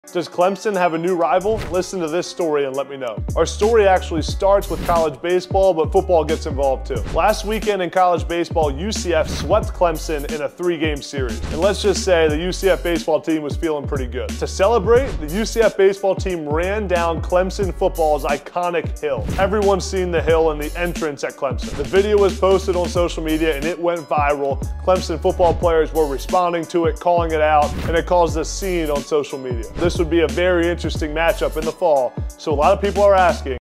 How Clemson Got Disrespected After Losing To UCF In Baseball
Does Clemson have a new rivalry brewing?🤔

Our story actually starts here with college baseball, but the football facilities get involved here too. Last weekend, UCF baseball swept Clemson in a 3-game series.

To celebrate, they ran down Clemson football team’s iconic hill.

We don’t know how they got in, but we do know this college football matchup would be interesting!